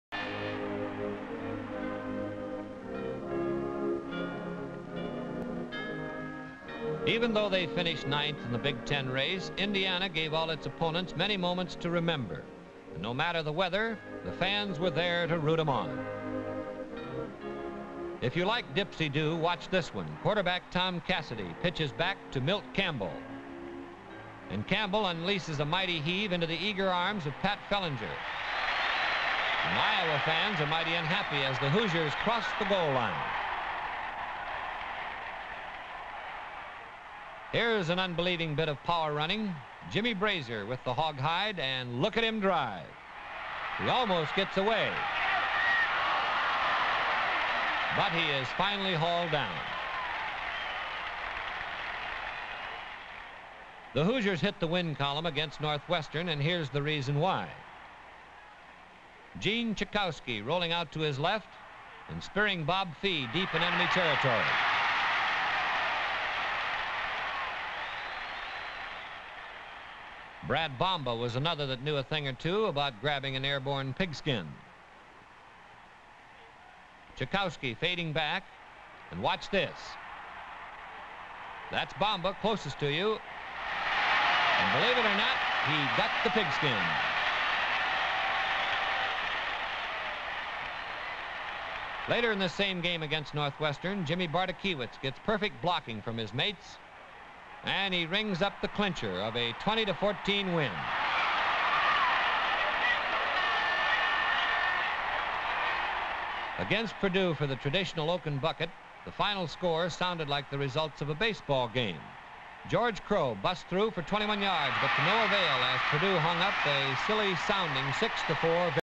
Big Ten Film Vault: 1955 Yearbook - Indiana Season Recap
A look at highlights of the 1955 Indiana football season.